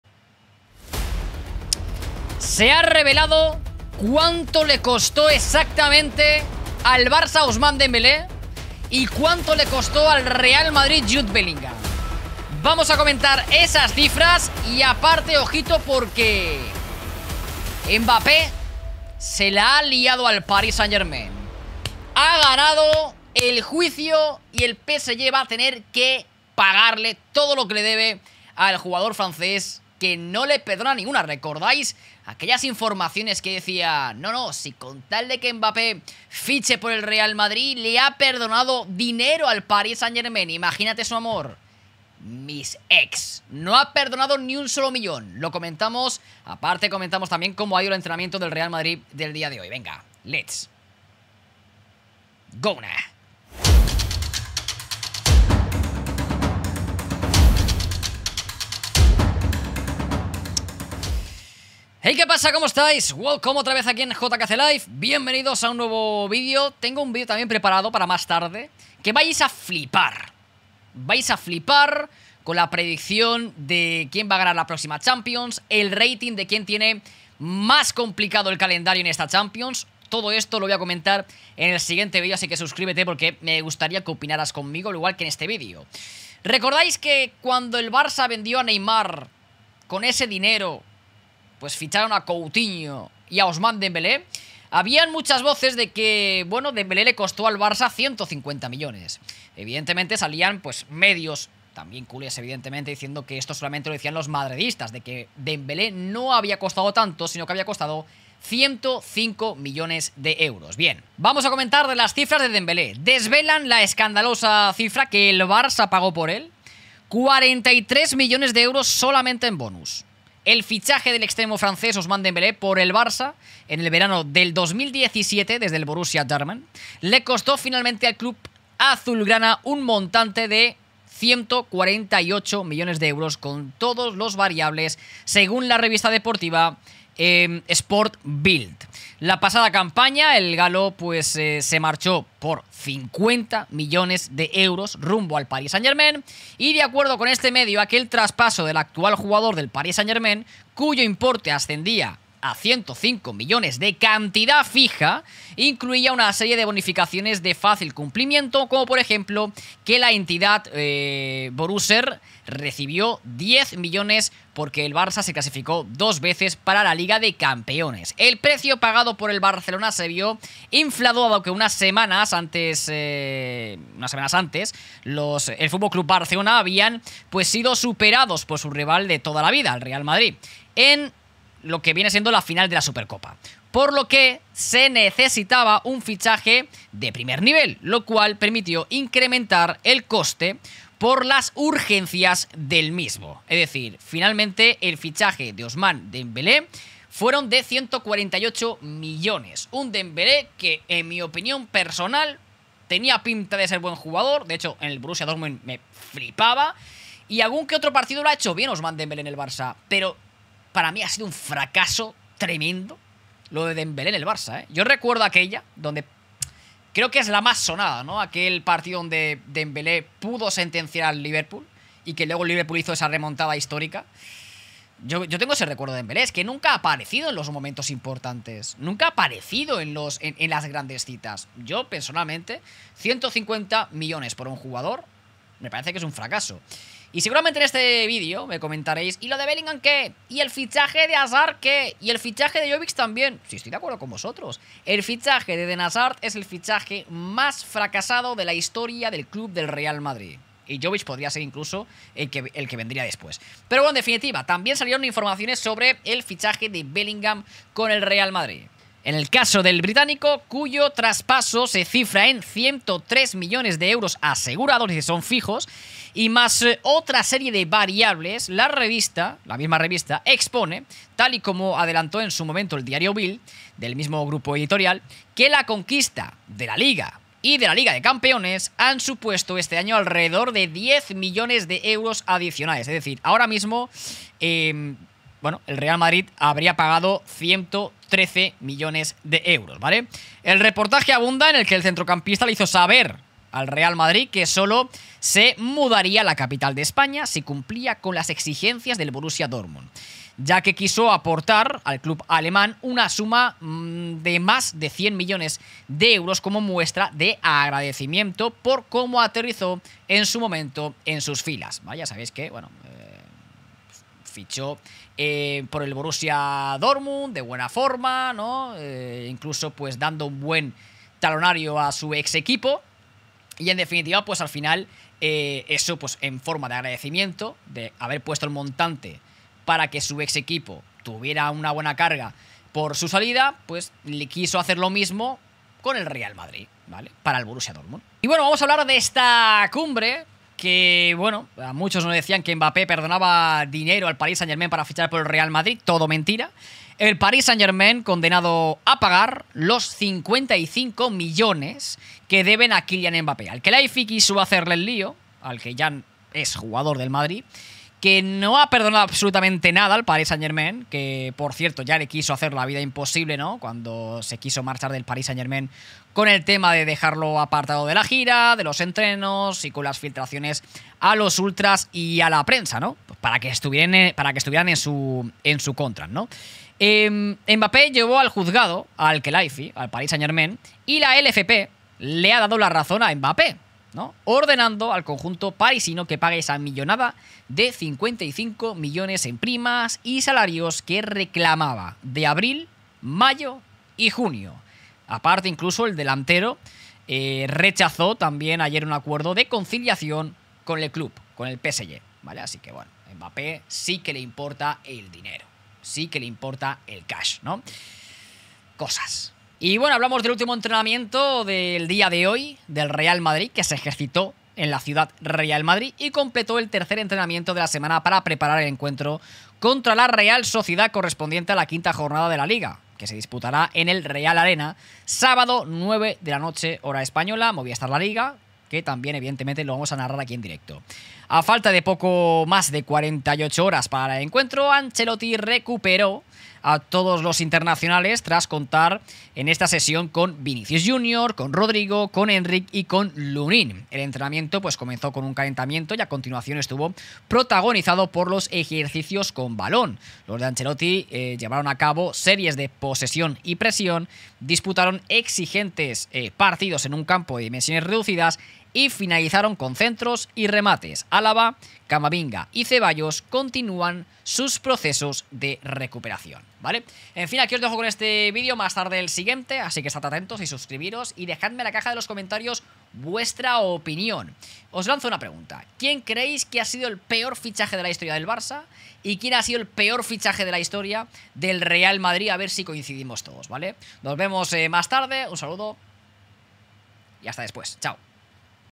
💥¡SE FILTRA el PRECIO REAL de BELLINGHAM al MADRID y DEMBÉLÉ al BARÇA! · ¡MBAPPÉ NO PERDONA NADA!
Filtran lo que le costó realmente Dembélé al Barça y Bellingham al Madrid.
- Mbappé gana el juicio contra el PSG. Eso de que Mbappé fichaba por el Madrid perdonando incluso dinero al PSG es mentira. OJO, que es absolutamente normal reclamar lo que es tuyo, pero vaya milongas se inventan.
- Así fue el entrenamiento de hoy del Madrid.